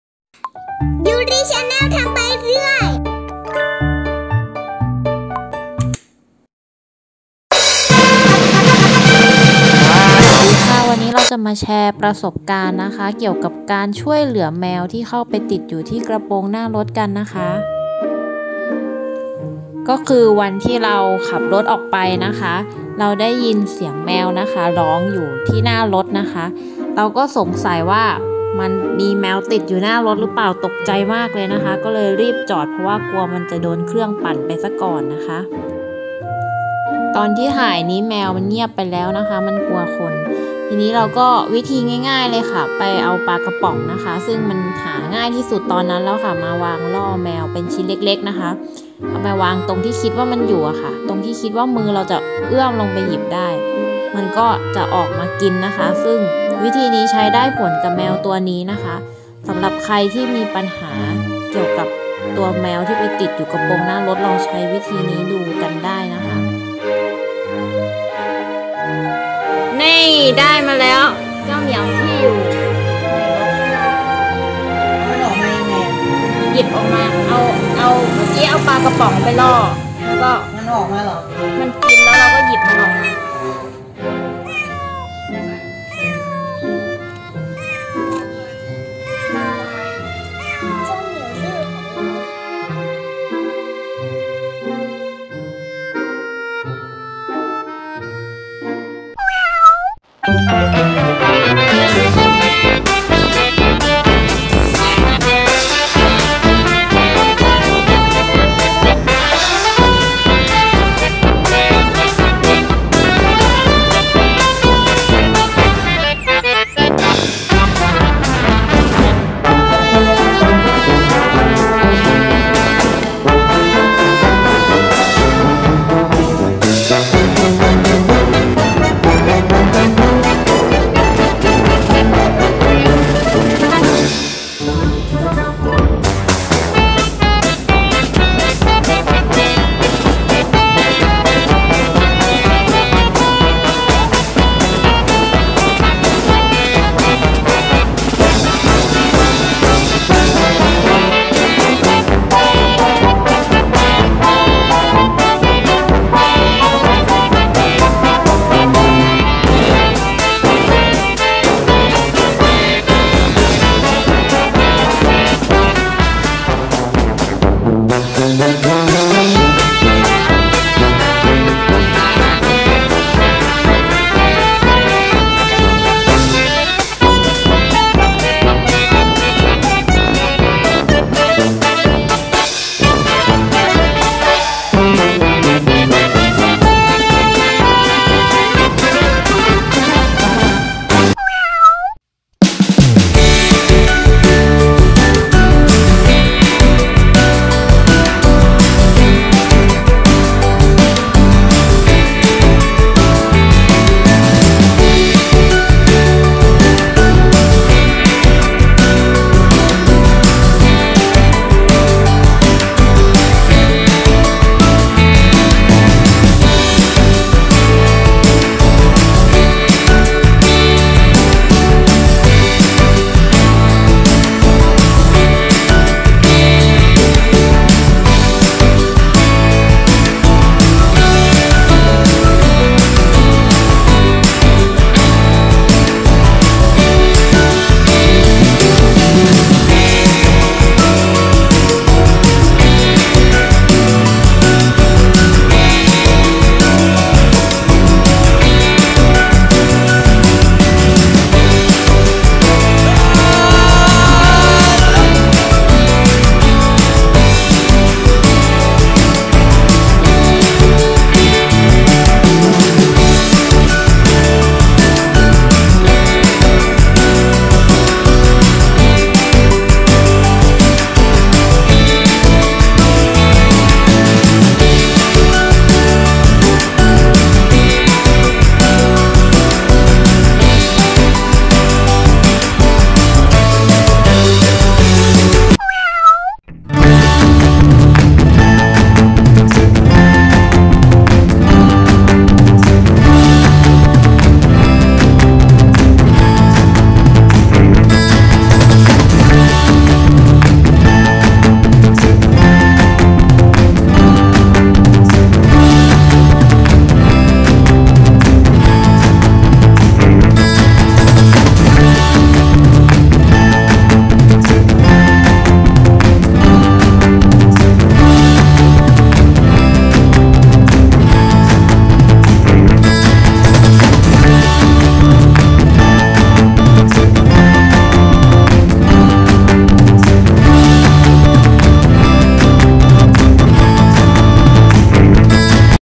แชร์วิธีช่วยชีวิตแมวที่ติดกระโปรงหน้ารถ | Yu clip
ใครหลายคนคงเคยมีประสบการณ์ แมวติดอยู่ฝากระโปรงรถ ถ้าได้ยินเสียงไม่ทัน อาจจะขับรถต่อจนแมวเด๊ดซะมอเร่ บรึ๋ยยย ไม่อยากจะคิด ... สำหรับคนที่ได้ยินเสียงเปิดรถออกมาดู แมวก็อาจจะหลบ ไม่ยอมออกมา "Yuri channel ทำไปเรื่อย" ก็มีประสบการณ์นี้  และเราช่วยชีวิตแมวได้สำเร็จ โดยการลองวางปลากระป๋องชิ้นเล็กๆ ล่อไว้ซอกที่แมวน่าจะอยู่ รอเวลา และเราก็จับเจ้าเหมียวได้จริง ไม่ต้องพึ่งพี่กู้ภัย เราเลยมาแชร์วิธีให้ทุกคนได้รับชมกัน และด้วยความน่ารักน่าชัง ขี้อ้อนของแมวเด็ก ตอนนี้บ้านเราก็กลายเป็นทาสแมวไปแล้วโดยปริยายค่าทุกค๊นนนน...🤣🐈❤️

ขอบคุณทุกคนที่ติดตามช่องเล็กๆของเราค่ะ
หวังว่าทุกคนจะชอบวิดิโอของเรา และสามารถติดตาม พูดคุยกับเราได้ที่ช่องทางเหล่านี้ค่ะ